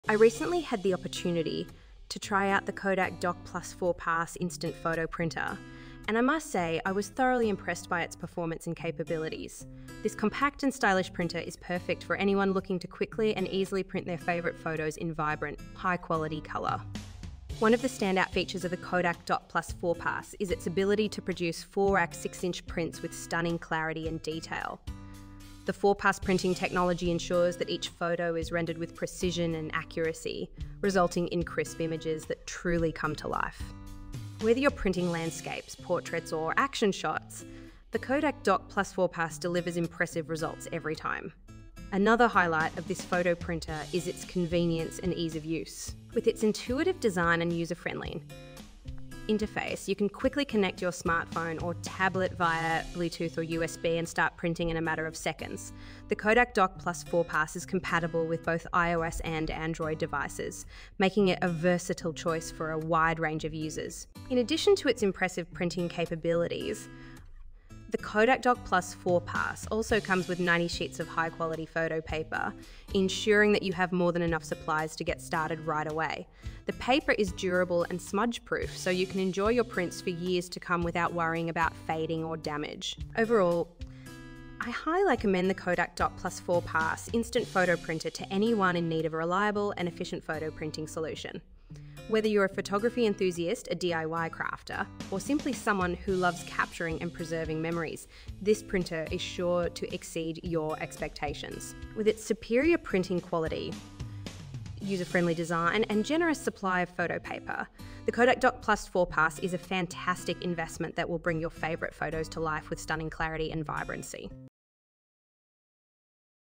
KODAK Dock Plus 4PASS Instant Photo Printer (4x6 inches) + 90 Sheets Review
I recently had the opportunity to try out the KODAK Dock Plus 4PASS Instant Photo Printer, and I must say, I was thoroughly impressed by its performance and capabilities. This compact and stylish printer is perfect for anyone looking to quickly and easily print their favorite photos in vibrant, high-quality color.

One of the standout features of the KODAK Dock Plus 4PASS is its ability to produce 4x6 inch prints with stunning clarity and detail. The 4PASS printing technology ensures that each photo is rendered with precision and accuracy, resulting in crisp images that truly come to life. Whether you're printing landscapes, portraits, or action shots, the KODAK Dock Plus 4PASS delivers impressive results every time.

Another highlight of this photo printer is its convenience and ease of use. With its intuitive design and user-friendly interface, you can quickly connect your smartphone or tablet via Bluetooth or USB and start printing in a matter of seconds. The KODAK Dock Plus 4PASS is compatible with both iOS and Android devices, making it a versatile choice for a wide range of users.

In addition to its impressive printing capabilities, the KODAK Dock Plus 4PASS also comes with 90 sheets of high-quality photo paper, ensuring that you have more than enough supplies to get started right away. The paper is durable and smudge-proof, so you can enjoy your prints for years to come without worrying about fading or damage.

Overall, I highly recommend the KODAK Dock Plus 4PASS Instant Photo Printer to anyone in need of a reliable and efficient photo printing solution. Whether you're a photography enthusiast, a DIY crafter, or simply someone who loves capturing and preserving memories, this printer is sure to exceed your expectations. With its superior printing quality, user-friendly design, and generous supply of photo paper, the KODAK Dock Plus 4PASS is a fantastic investment that will bring your favorite photos to life with stunning clarity and vibrancy.